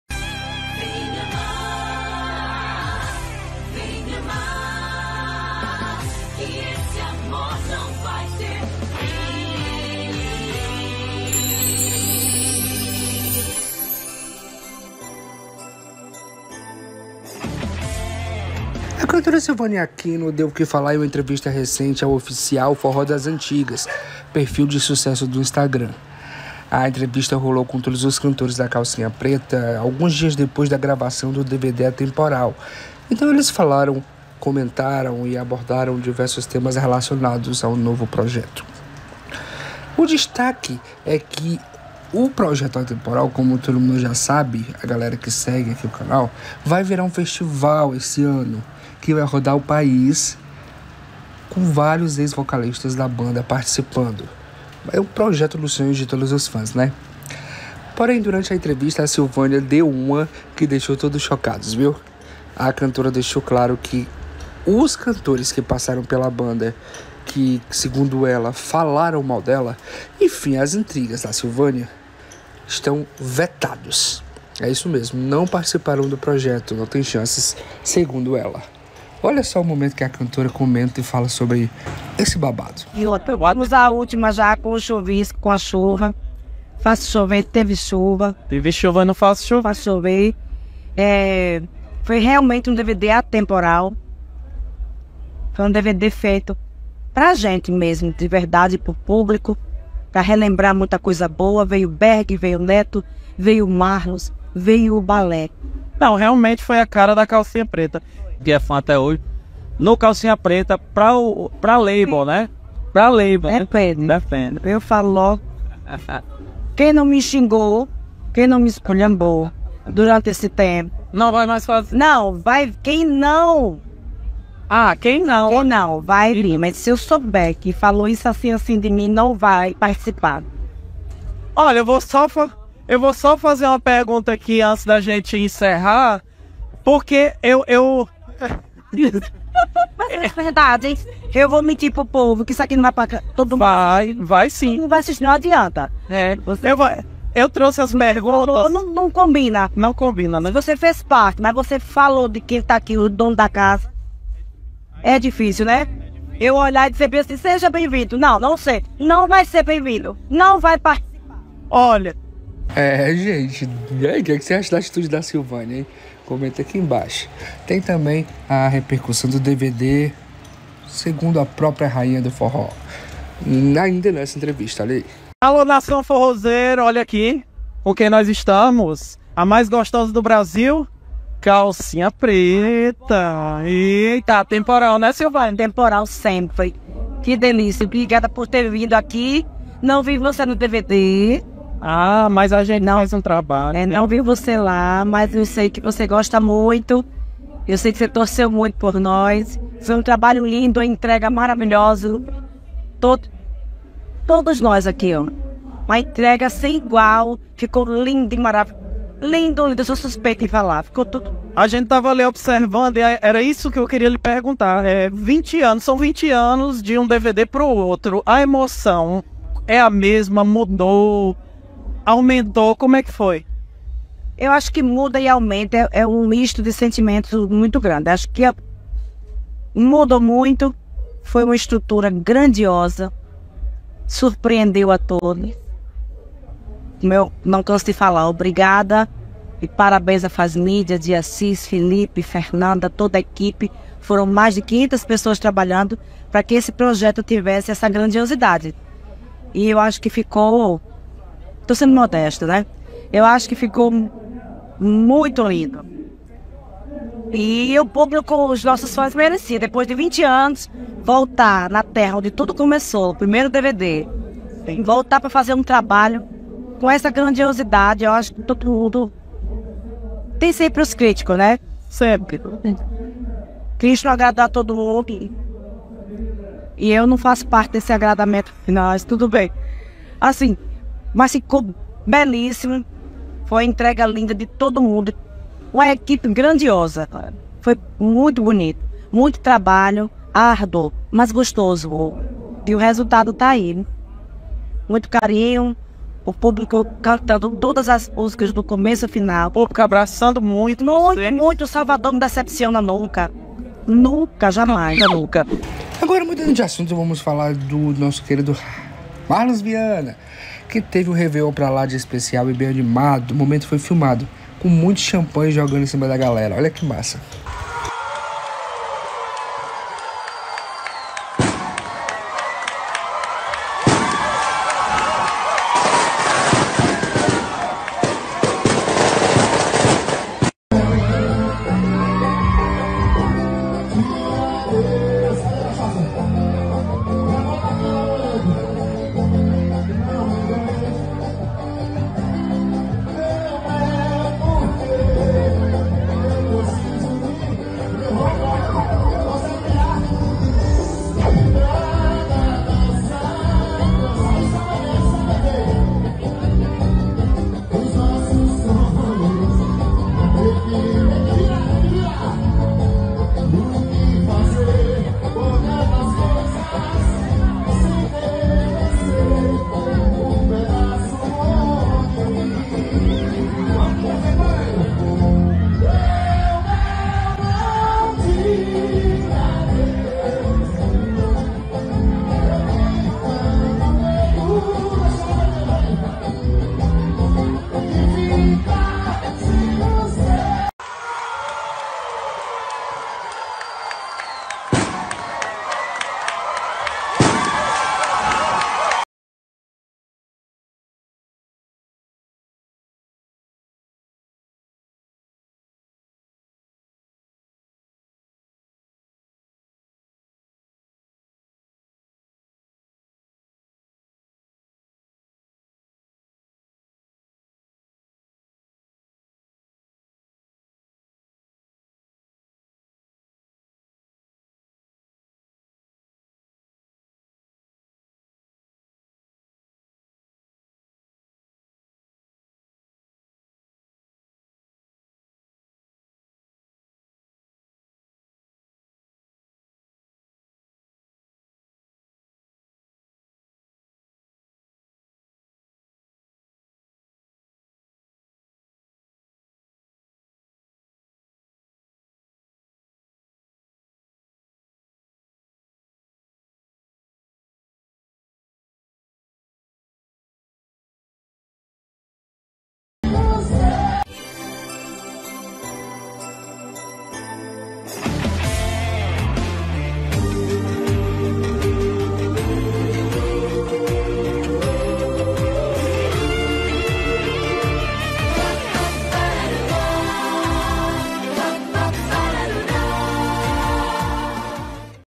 Silvânia Aquino NÃO PERDOA EX CANTORES, PROIBE d SUBIR NO PALCO D CALCINHA PRETA e MANDA, "AQUI NÃO"
Silvânia Aquino NÃO PERDOA EX CANTORES E PROIBE de SUBIR NO PALCO DA CALCINHA PRETA NOVAMENTE, "AQUI NÃO". Saiba tudo

Facebook Grupo : Portal Cearabelha
Facebook Página : Portal Cearabelha
Periscope : Cearabelha
Google+ : Cearabelha